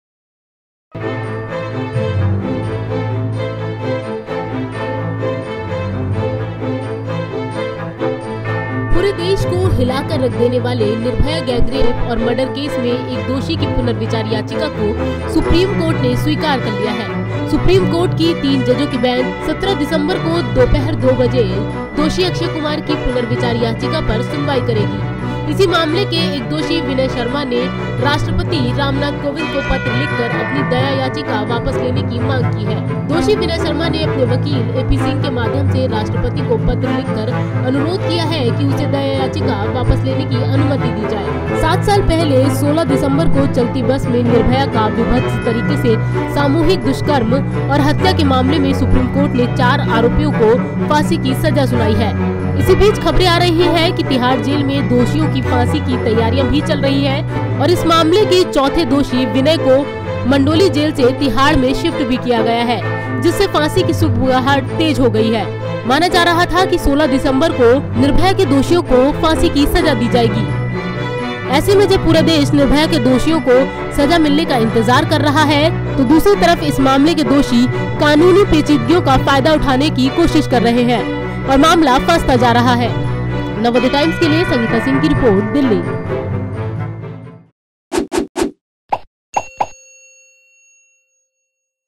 Nirbhaya के दोषियों की फांसी में और होगी देरी, दोषी अक्षय की Review Petition पर SC करेगा सुनवाई
Nirbhaya के दोषियों की फांसी में अभी और होगी देरी, दोषी अक्षय की पुनर्विचार याचिका पर SC करेगा सुनवाई